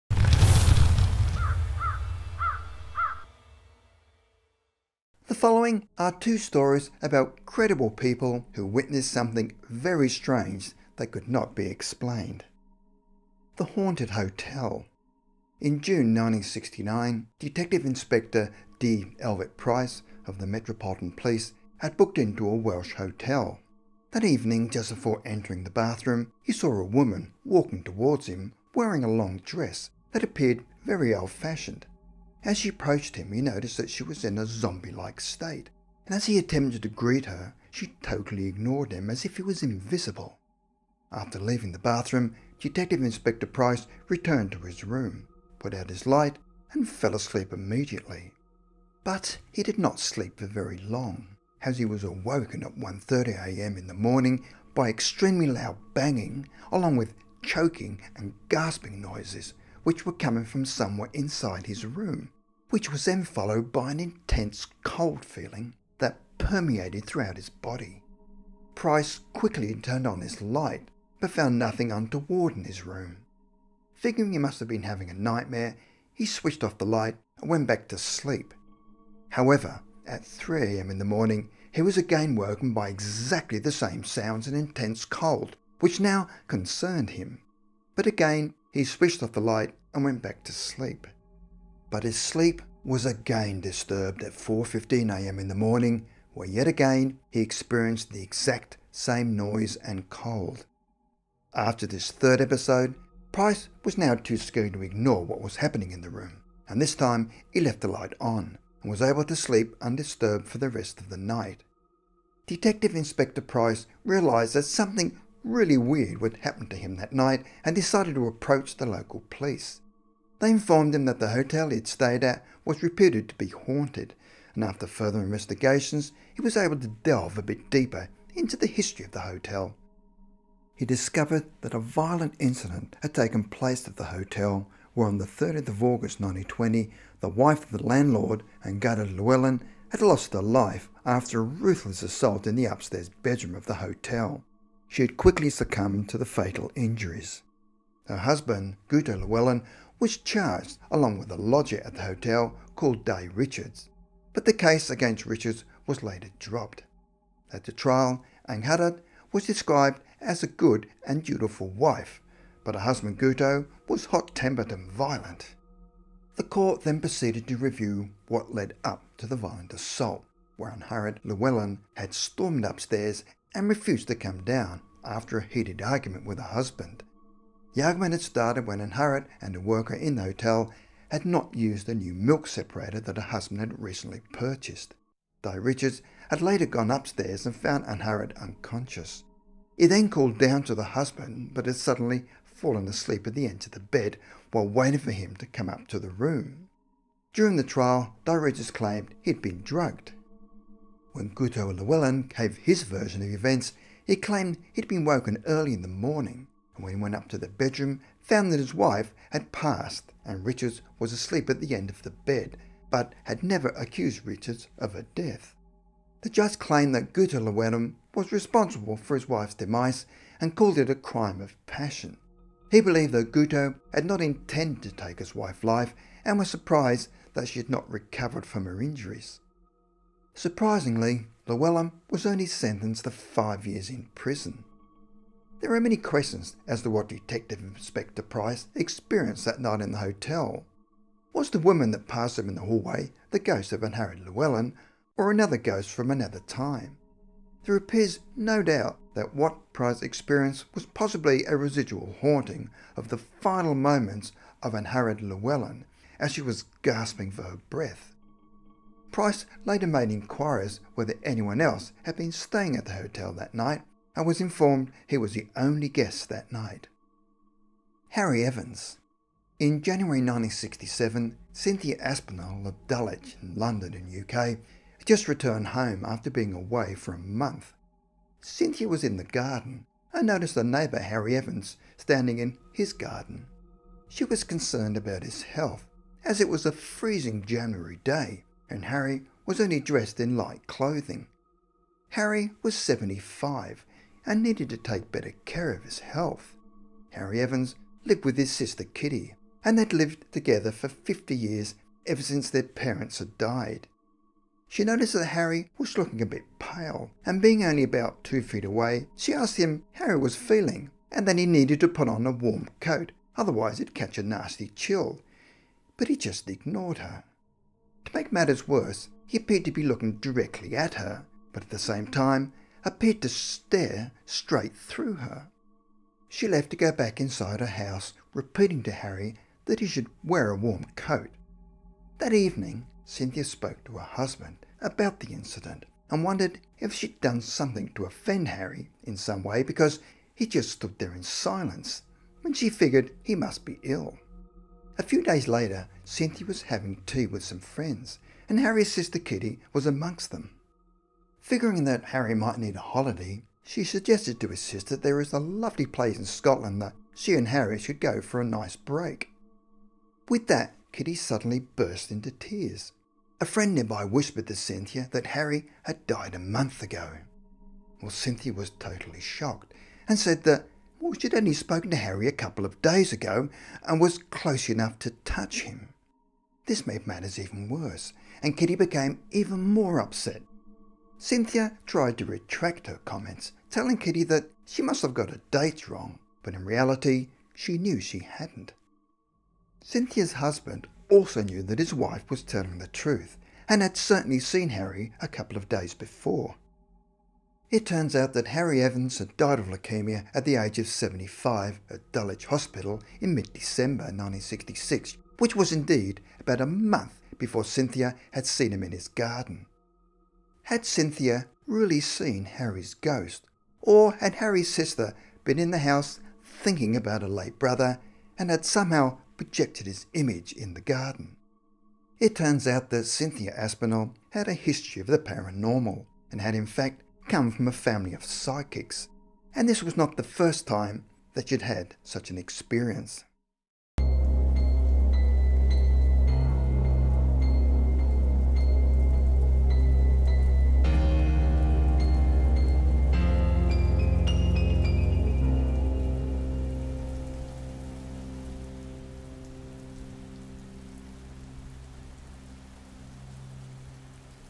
Unsolved HAUNTINGS That Will Make You Think Twice
The following are two stories about credible people who witnessed something very strange that could not be explained.

Outro Credits:
Greenscreen Thunder Storm HD